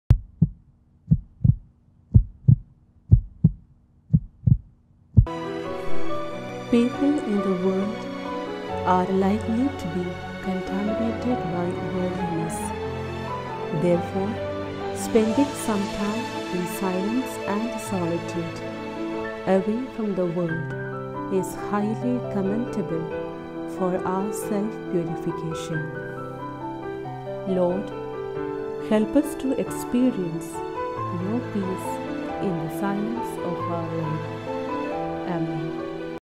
Heart Beats # Mar Augustine Kandathil
A reflection on the maxims of Mar Augustine Kandathil